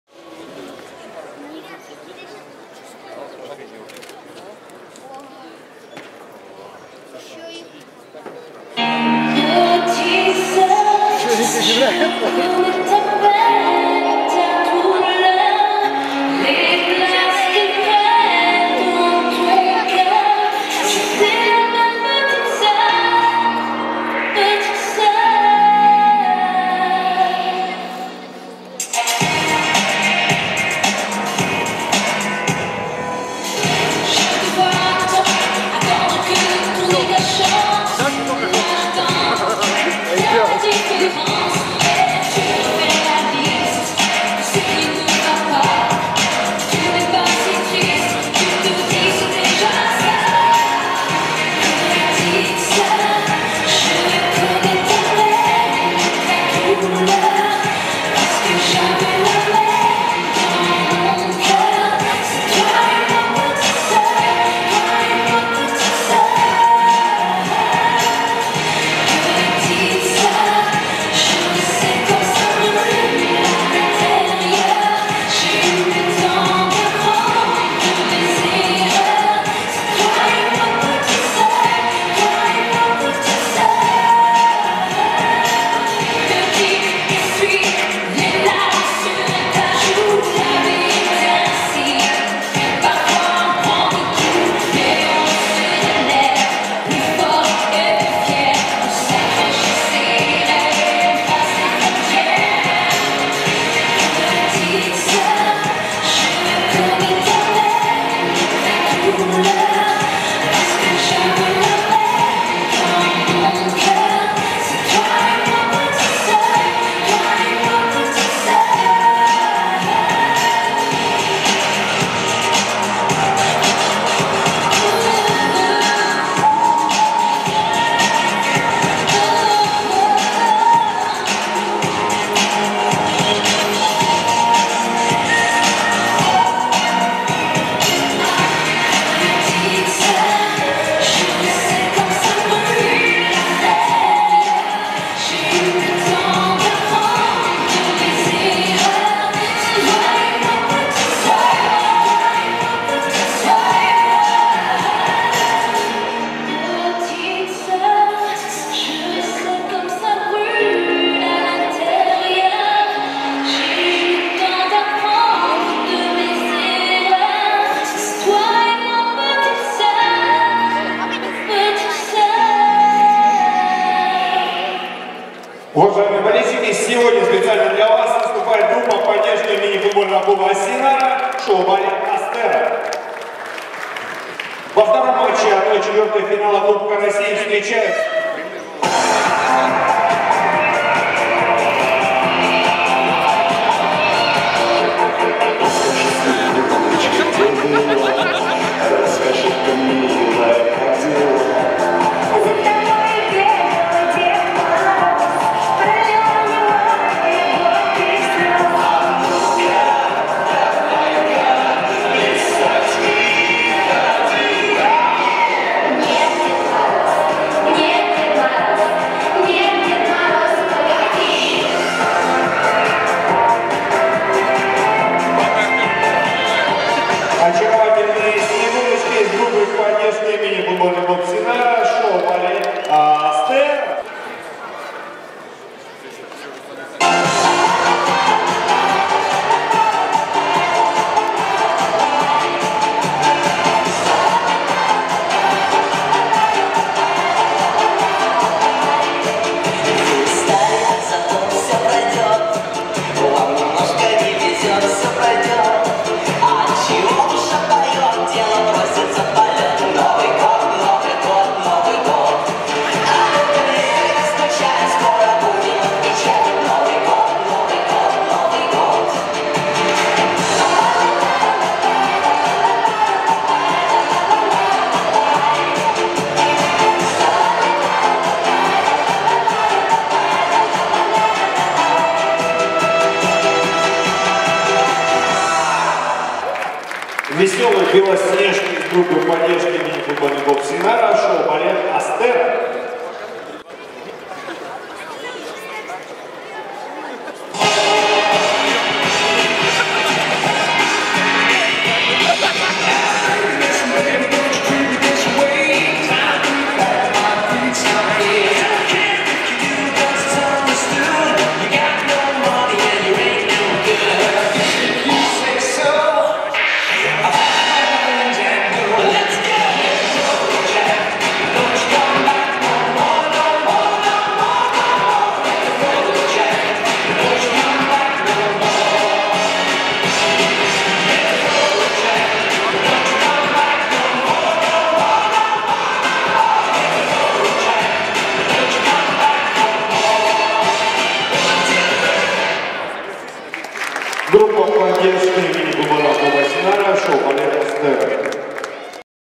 Show Ballet Astera: Cool Beauty and Artistry on Stage Female Cheerleading
Astera Show Ballet: The Perfect Combination of Coolness and Elegance. Astera's unique blend of classical ballet and show ballet creates the perfect combination of coolness and elegance in every routine. Experience the magic of Astera as their talented dancers bring their energy and passion to the stage in this stunning display of artistry.

You won't forget this amazing performance by a female cheerleading squad anytime soon! Watch as they deliver a routine that is truly unforgettable!